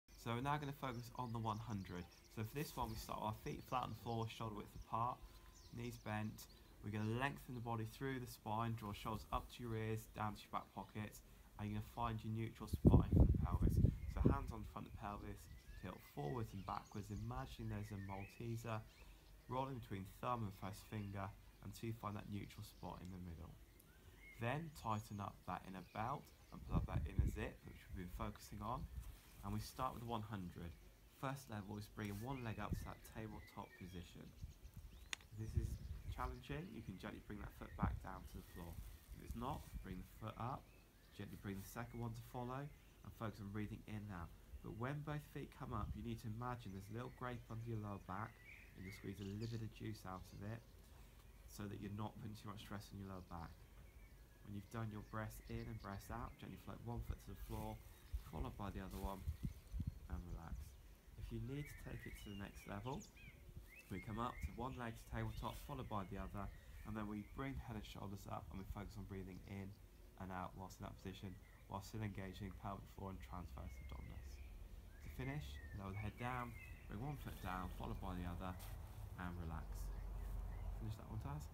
Pilates One Hundred
A short video explaining the One hundred Pilates exercise and the Lengthening lumbar spine exercise too